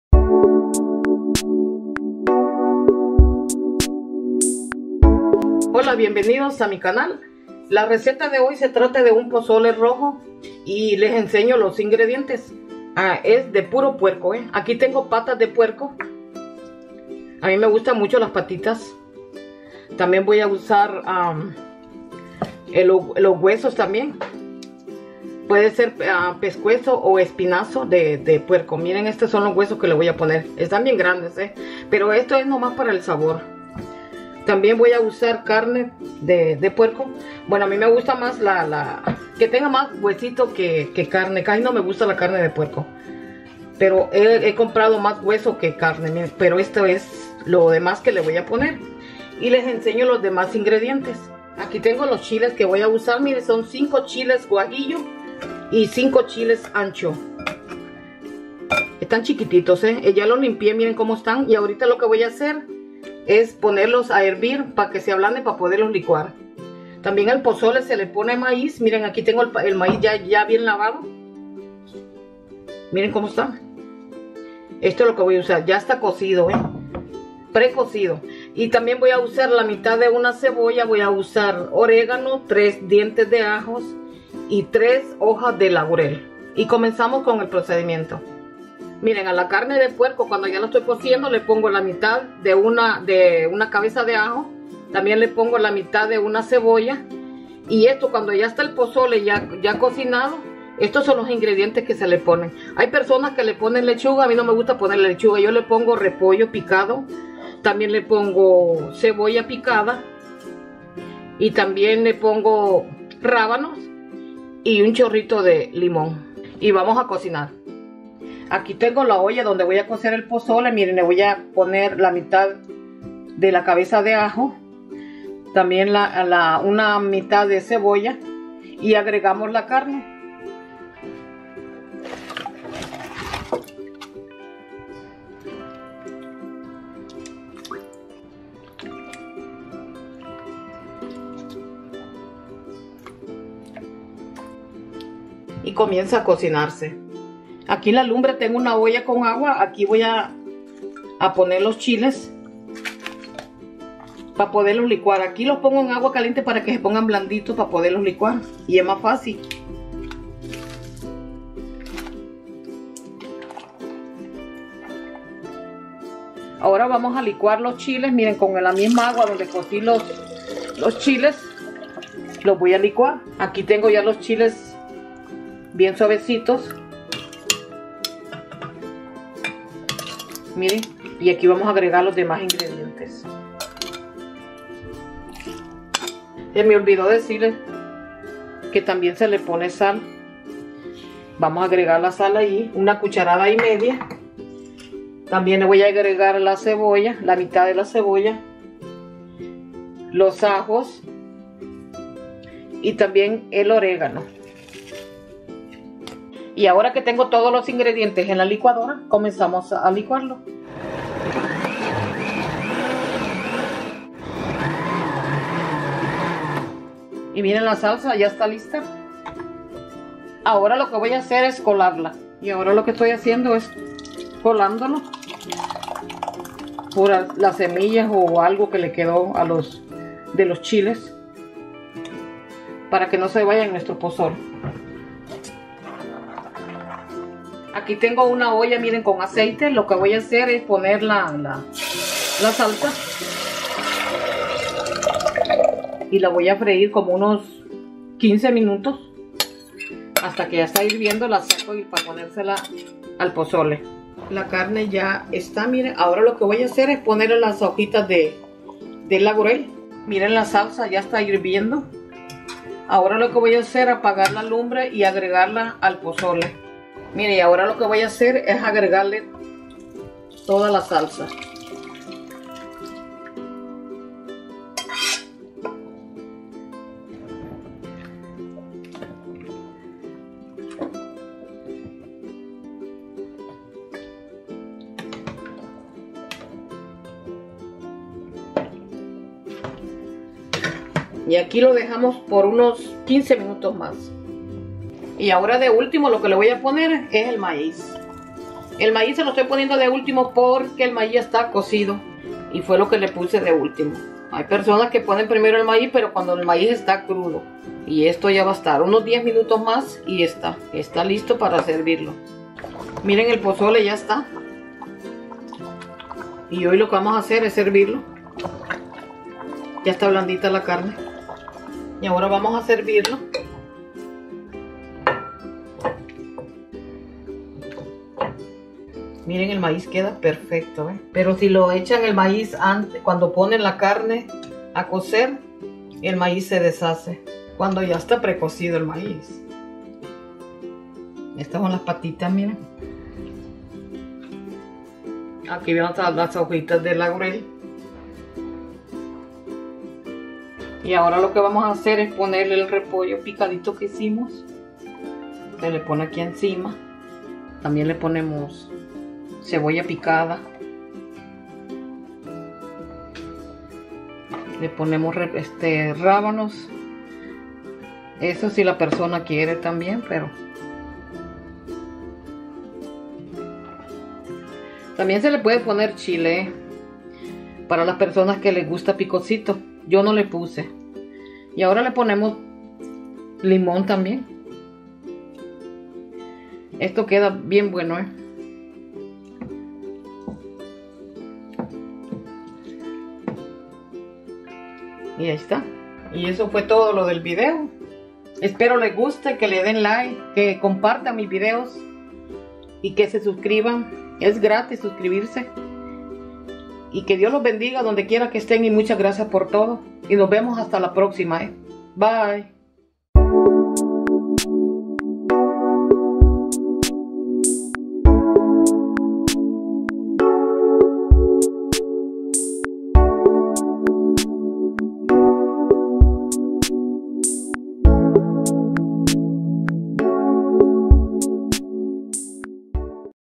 Pozole Rojo De Puerco/Red Pork Pozole
ingrediente:
5 Patas de Puerco
3 libras de Carne de Puerco
3 libras de Hueso
5 Chiles Guajillo
5 Chiles Ancho
1/2 Maíz
1 Cebolla
2 cdas de Oregano
3 Ajos
3 Hojas de Laurel
3 cdas de Sal

ingredient:
5 Pork Legs
3 pounds of Pork Meat
3 pounds of Bone
5 Guajillo Chiles
5 Ancho Chiles
1/2 Corn
1 Onion
2 tbsp Oregano
3 Garlic
3 Bay Leaves
3 tbsp salt